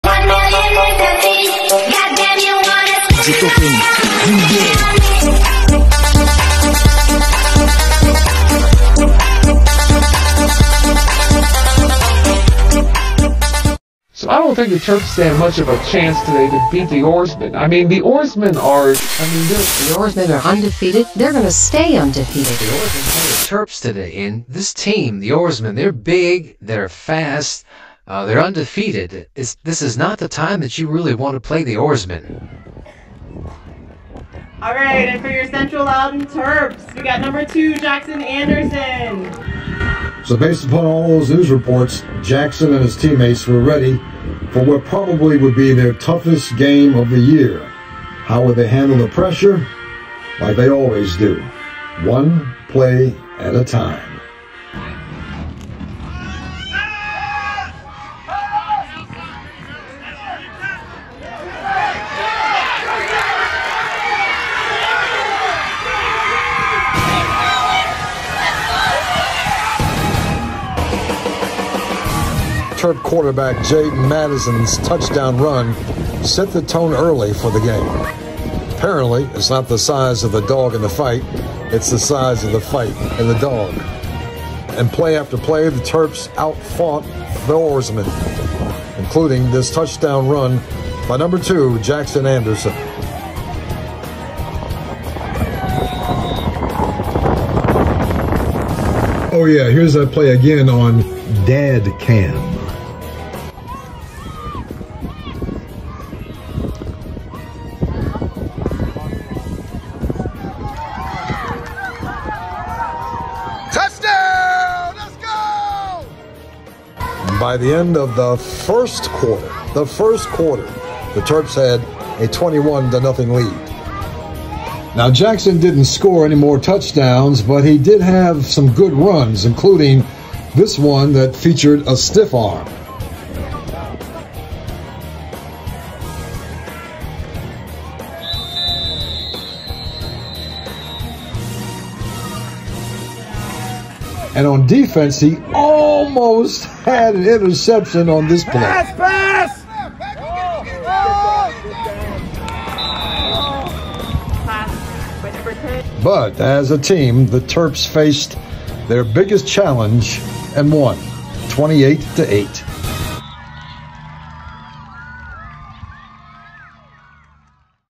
Jaxson and the Terps face the vaunted OARSMEN!!
If you listened to the pre-game hype about the OARMEN, you probably wouldn't want to play the game at all.    They were from upper Loudoun County where only the biggest and the best were recruited. and they were undefeated.  they were.